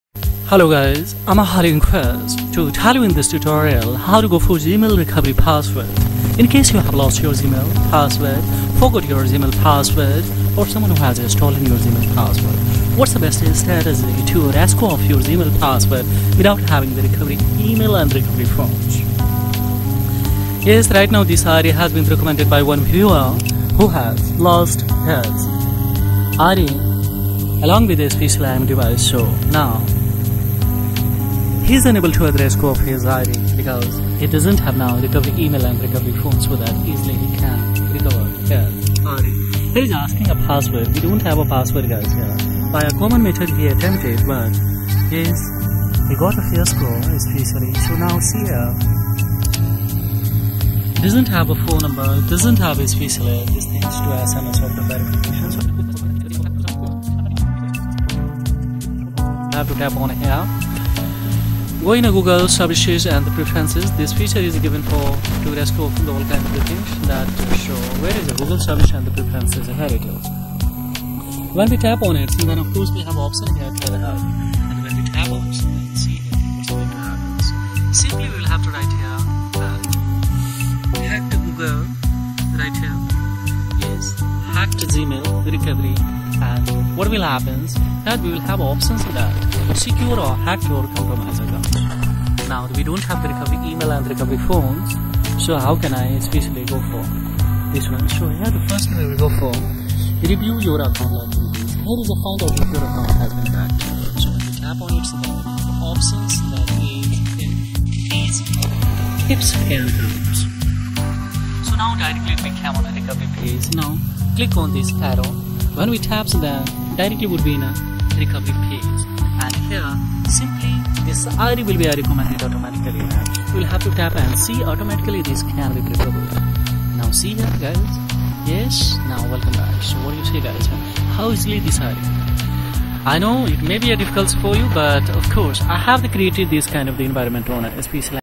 gmailrecovery ForgotPassword GmailHelp RecoverGmail forgotgmailpassword googleaccount
Forgot your Gmail password and can't access your recovery email or phone number? Don't worry! In this updated 2025 beginner's guide, we’ll walk you through step-by-step methods to reset your Gmail password without needing a recovery email or phone.

✅ What you'll learn in this video:

How to reset Gmail using Google’s help center
Alternative verification options
Identity verification tips to recover access
What to do if you’re completely locked out